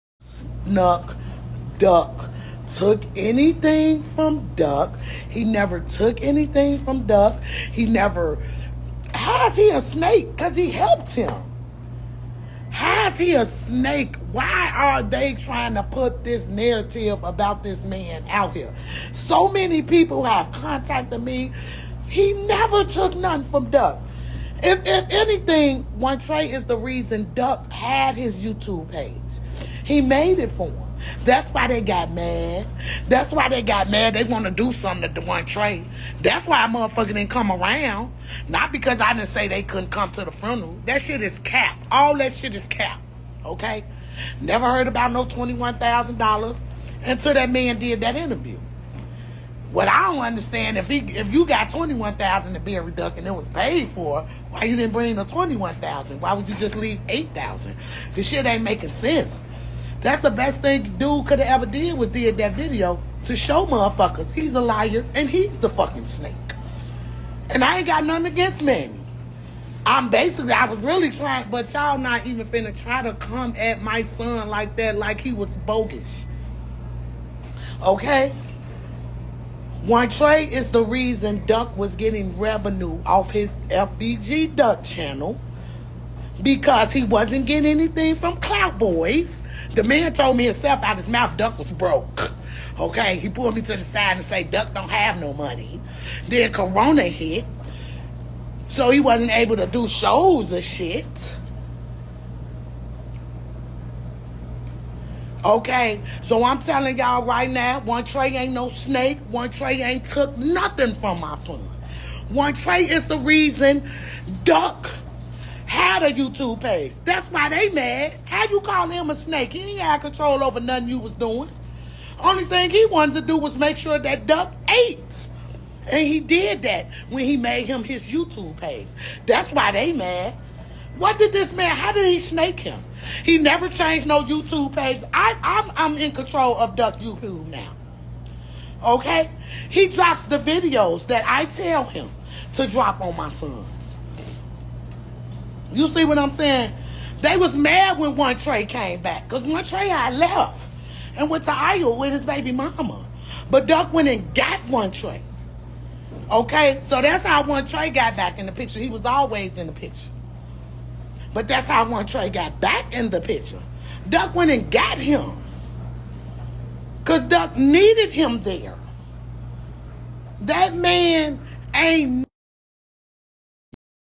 fbg duck mom go's off on ducks manager for saying bcr one tray stole
Manny ducks old manager and fbg cash and young's father try to say bcr one tray stole duck money momma duck say that's not true credit to momma duck and x-man reeko go follow them
in use of the fair act this is a public video respectfully